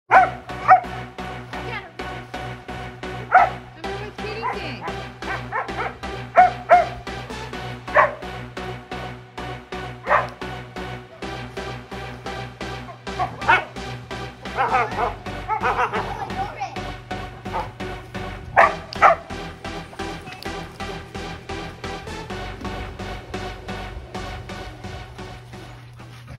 Cute Dog Teasing Her BFF On The Trampoline| Ember The English Springer Spaniel
Ember and Katie the English Springer Spaniels love playing together. Today Ember teases Katie from the trampoline because Katie can't get her.
These girls have so much fun together and are definitely Best Friends Forever!
Love
Ember and Katie